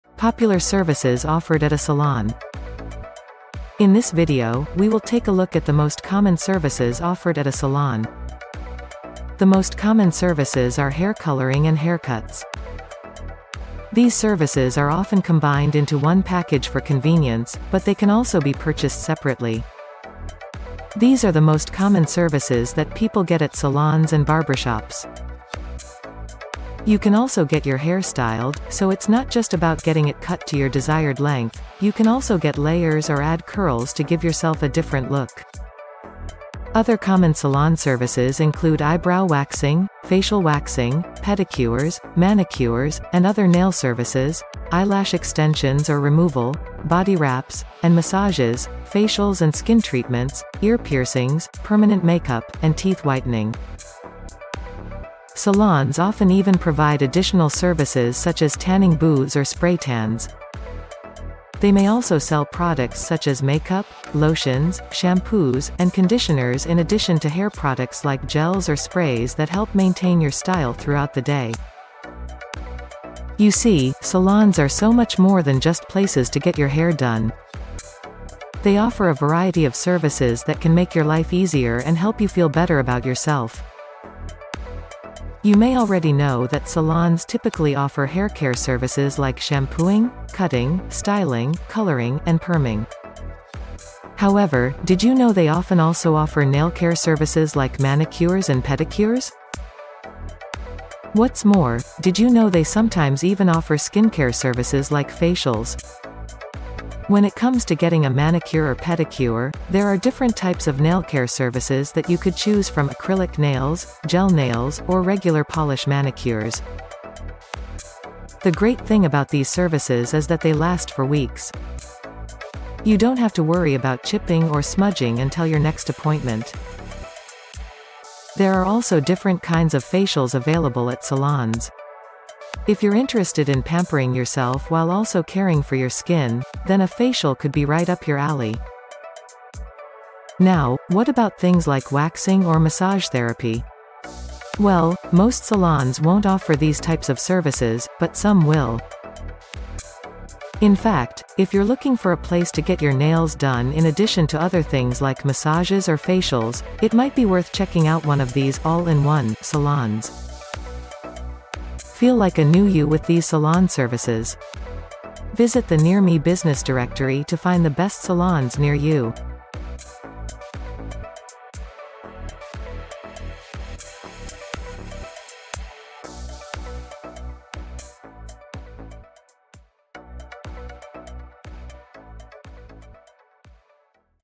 Popular Services Offered at a Salon
Chapter
00:08 The Most Popular Services That are Offered at a Salon

In this video, we will take a look at the most common services offered at a salon.

The most common services are hair coloring and haircuts. These services are often combined into one package for convenience, but they can also be purchased separately. These are the most common services that people get at salons and barbershops. You can also get your hair styled, so it's not just about getting it cut to your desired length, you can also get layers or add curls to give yourself a different look.

Other common salon services include eyebrow waxing, facial waxing, pedicures, manicures, and other nail services, eyelash extensions or removal, body wraps, and massages, facials and skin treatments, ear piercings, permanent makeup, and teeth whitening.

Salons often even provide additional services such as tanning booths or spray tans. They may also sell products such as makeup, lotions, shampoos, and conditioners in addition to hair products like gels or sprays that help maintain your style throughout the day.

You see, salons are so much more than just places to get your hair done. They offer a variety of services that can make your life easier and help you feel better about yourself.

You may already know that salons typically offer hair care services like shampooing, cutting, styling, coloring, and perming. However, did you know they often also offer nail care services like manicures and pedicures? What’s more, did you know they sometimes even offer skin care services like facials?

When it comes to getting a manicure or pedicure, there are different types of nail care services that you could choose from acrylic nails, gel nails, or regular polish manicures. The great thing about these services is that they last for weeks! You don't have to worry about chipping or smudging until your next appointment!

There are also different kinds of facials available at salons. If you're interested in pampering yourself while also caring for your skin, then a facial could be right up your alley!

Now, what about things like waxing or massage therapy? Well, most salons won't offer these types of services, but some will! In fact, if you're looking for a place to get your nails done in addition to other things like massages or facials, it might be worth checking out one of these "all-in-one" salons.

Feel like a new you with these salon services!
Visit the NearMe business directory to find the best salons near you!